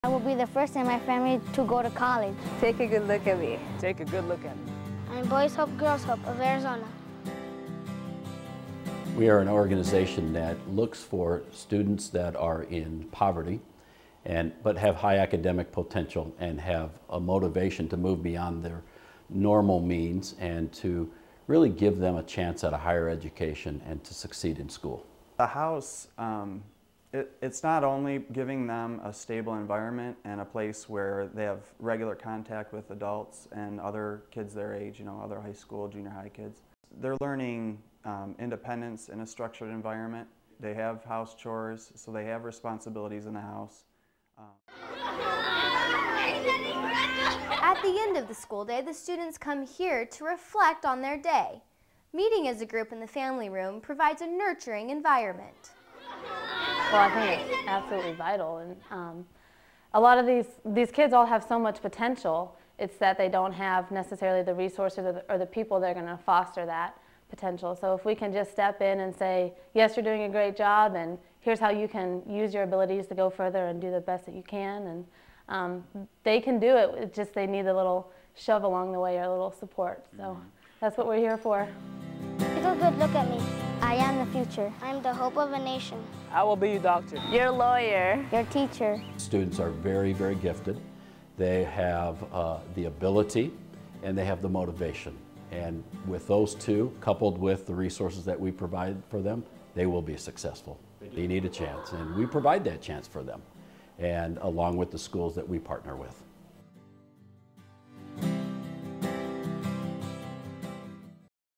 Boys Hope Girls Hope Arizona
Boys Hope Girls Hope Arizona gives children the opportunity to live in a house with counselors and attend schools in the Phoenix area.